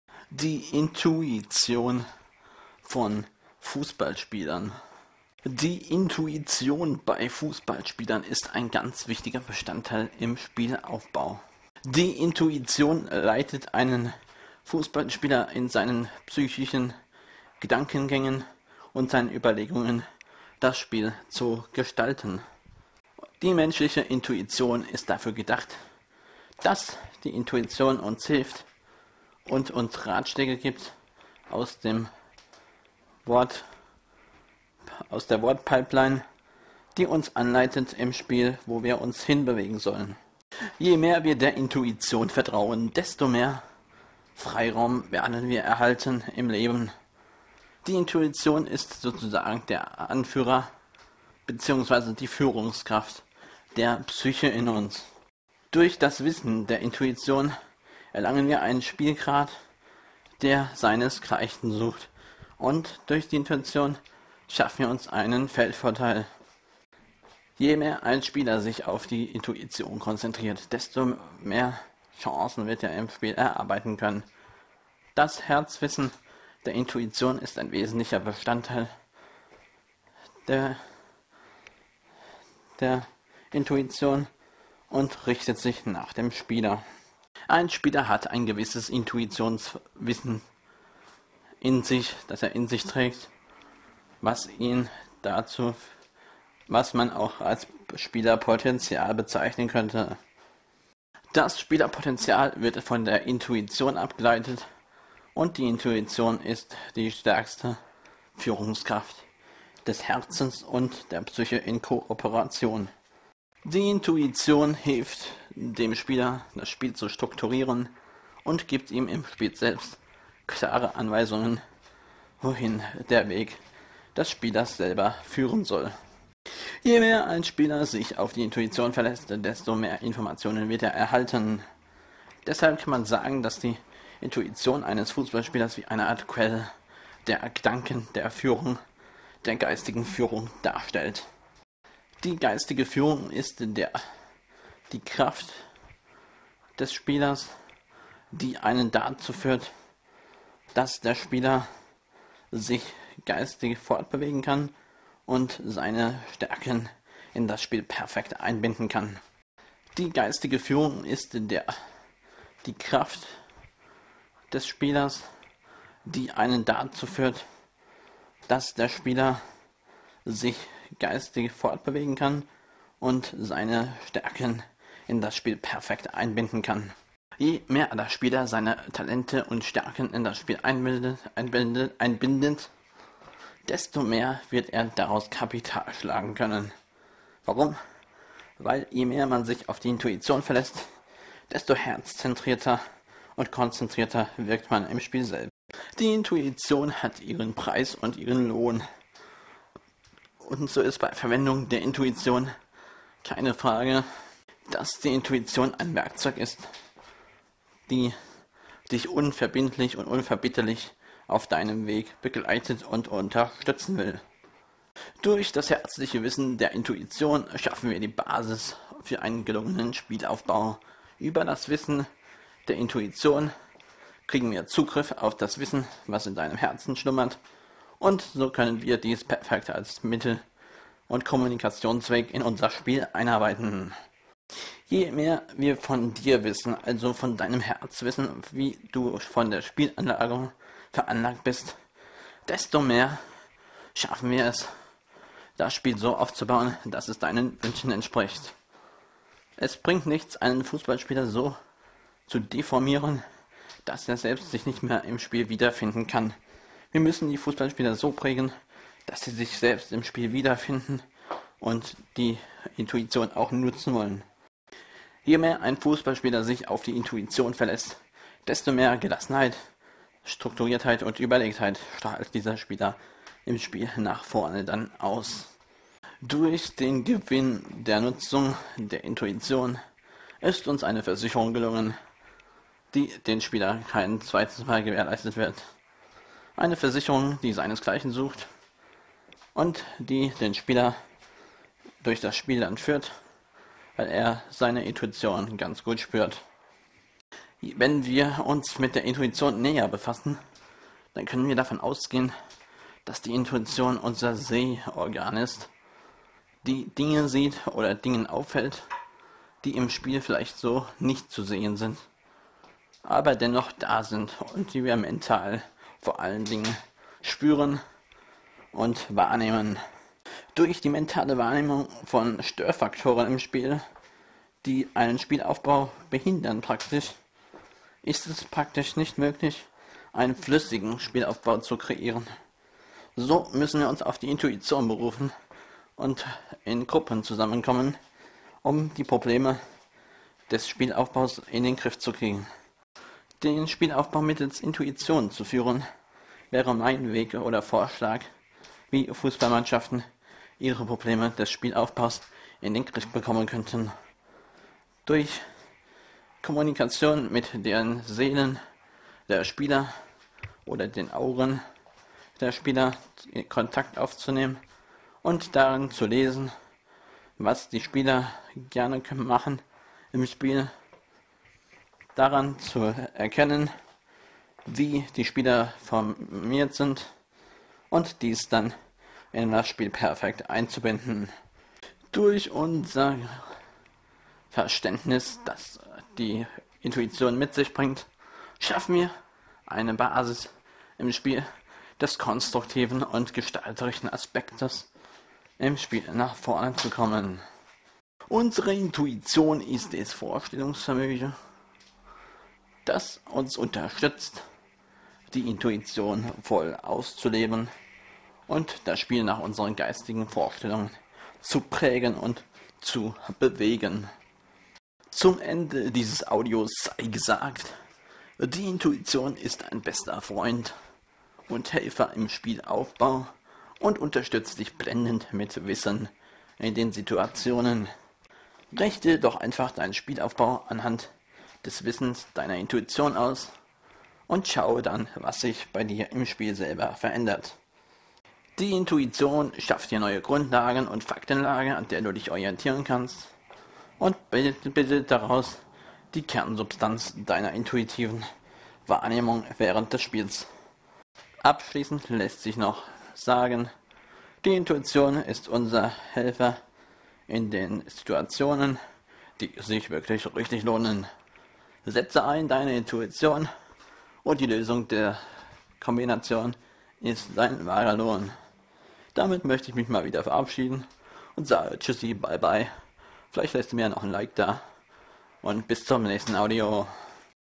[Herzintuition] Das Wissensaudio zum Thema Spielerintuition!
Fußballspieler haben alle Intuitionen. Ich zeige auf, wie Sie mit Ihrer Intuition mehr anfangen können!